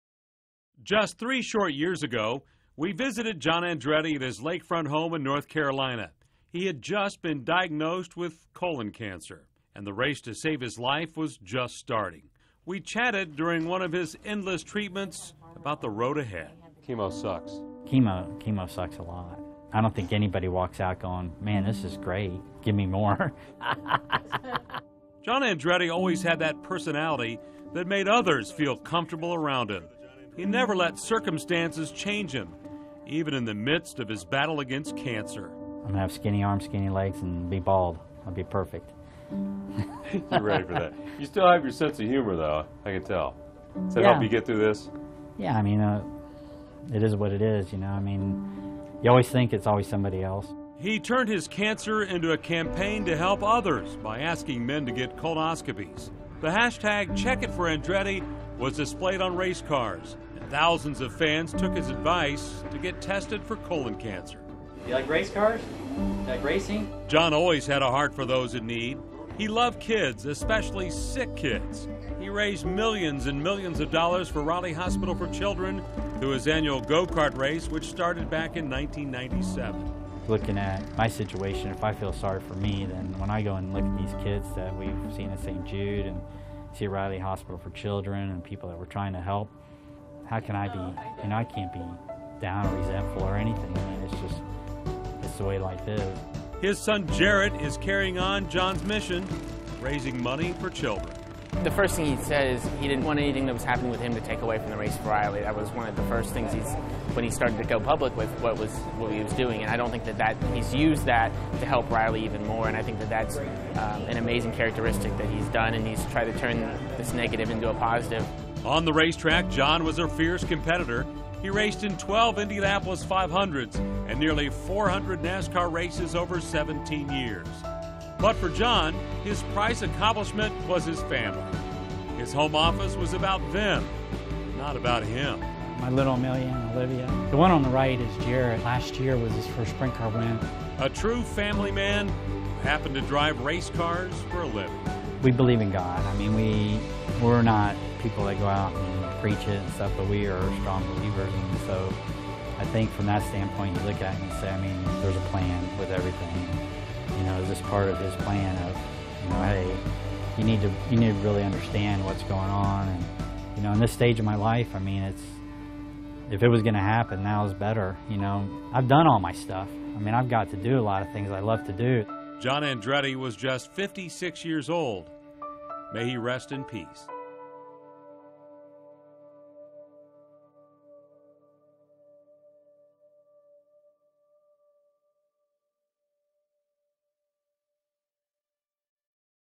Obituary for race car driver John Andretti, 1963-2020
IndyCar driver John Andretti died at the age 56 after a long battle with colon cancer. Andretti also drove in NASCAR and CART. The death of the driver hit the Indianapolis racing community hard.